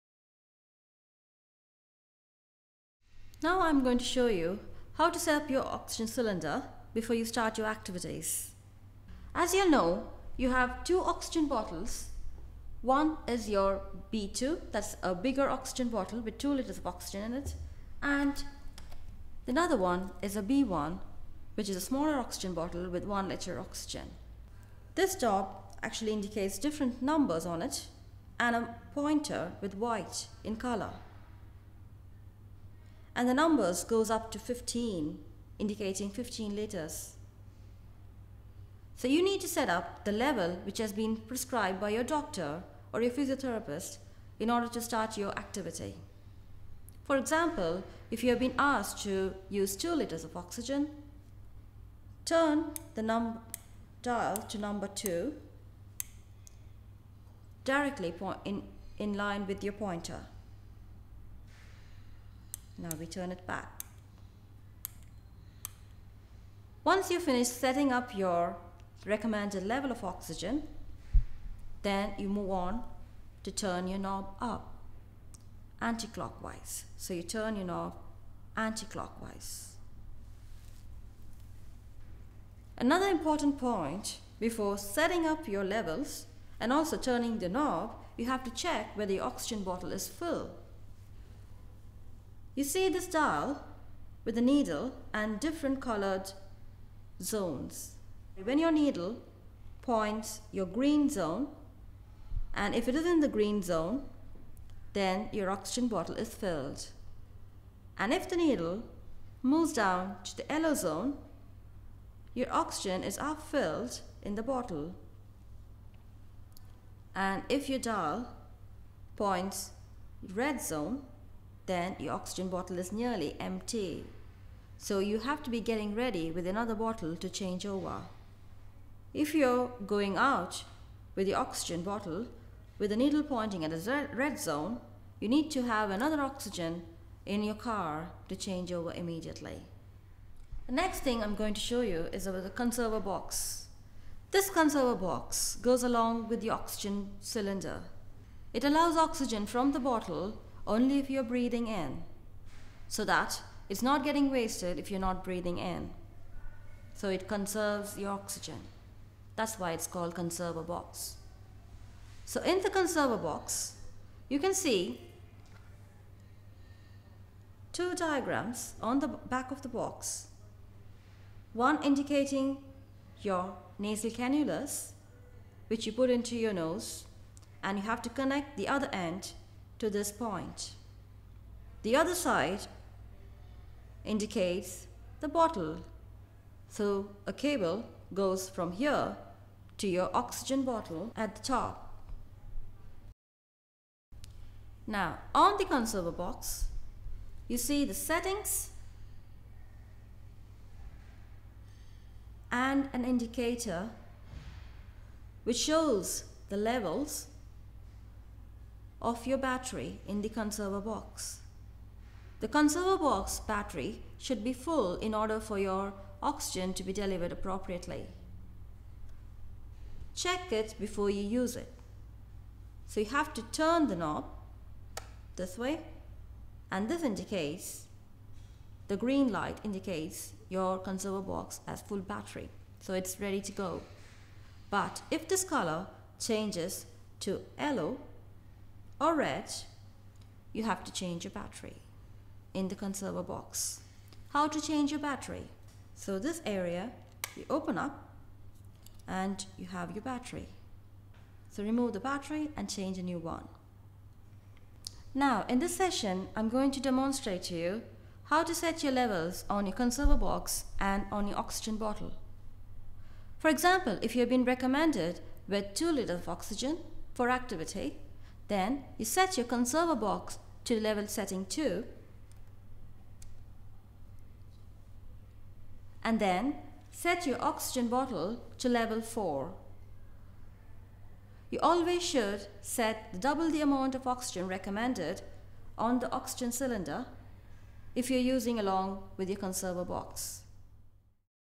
13 Portable Oxygen Tank for respiratory illness.mp4
The Irish Thoracic Society explain the usage of a portable oxygen tank for patients with acute or chronic respiratory illnesses and diseases and respiratory distress.